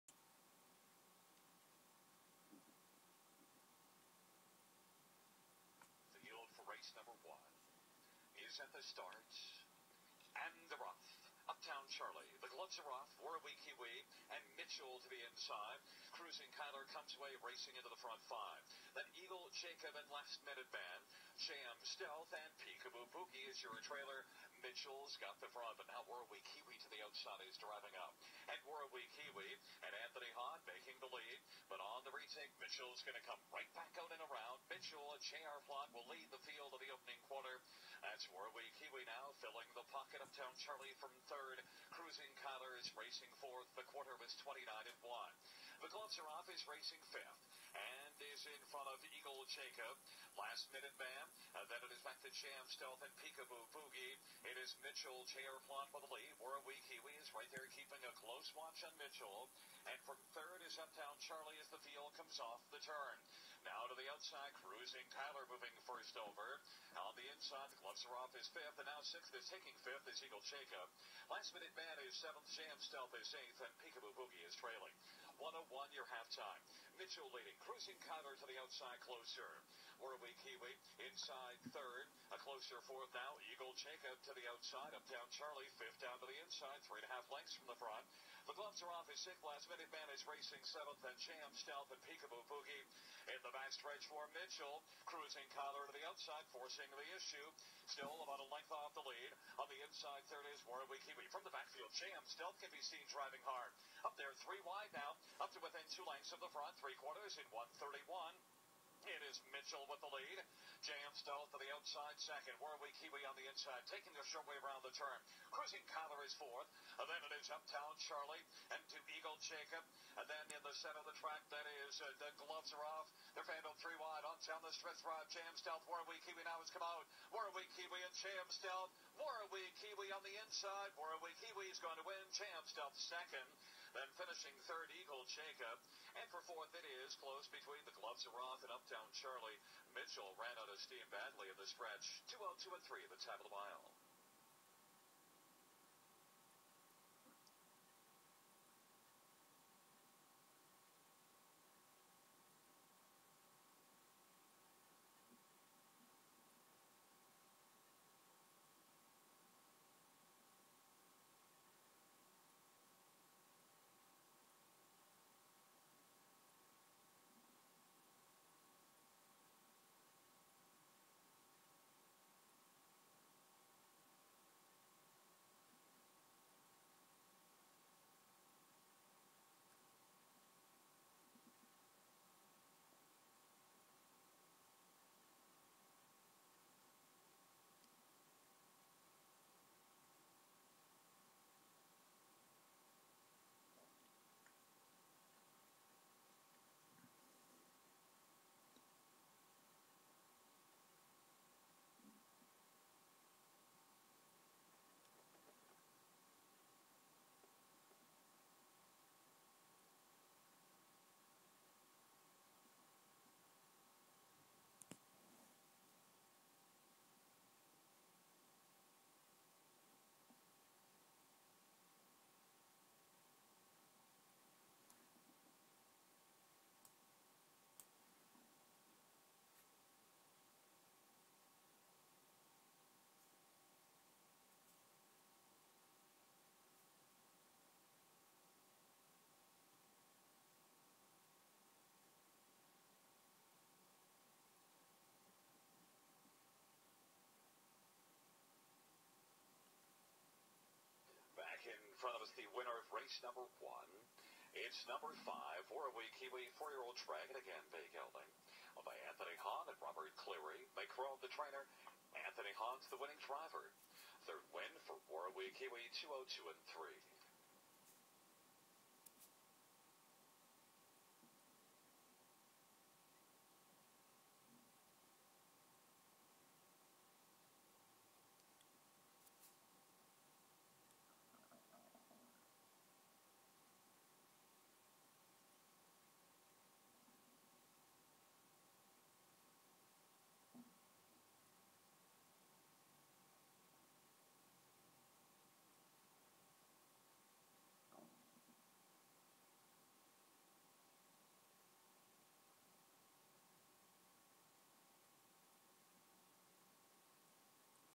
WARAWEE KIWI - Race 1, Flamboro Downs, Jan 2, 2009
RACE 1
1 MILE, PACE, PURSE $4,700.
COLTS & GELDINGS CLAIMING $8,000. ALLOWANCES.
FOR NW 2 RACES OR $15,000 LIFETIME ALLOW.

Time: 29.1, 1:01, 1:31, 2:02.3 (Temperature: -12, Condition: FT, Variant: 3)

1st  Warrawee Kiwi       (b,g,4 - Dragon Again-La Bella Vita-Life Sign)
                         Owner: Anthony P Haughan-Robert Cleary,Cambridge,ON
2nd  J Ms Stealth        (b,g,5 - Mach Three-Oprah Seelster-Threefold)
                         Owner: Wilbert R Weatherall,Markdale,ON
3rd  Eagle Jacob         (br,g,5 - The Panderosa-Janice Cambest-Cambest)
                         Owner: Sherry L Berger,Upton,MA-Stephen L Doyle,Dundas,ON